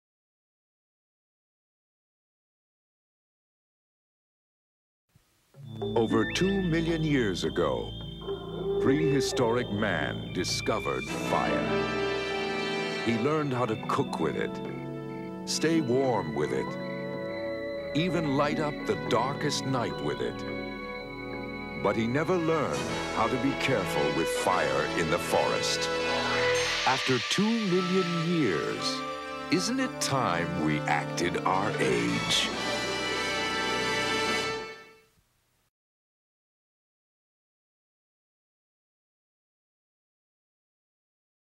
Smokey Bear - 2 Million Years (1983)
Smokeybear.com  It is said that humans discovered fire 2 million years ago, yet we are still as careless with it as ever. Please visit Smokeybear.com to find out how YOU can prevent wildfires.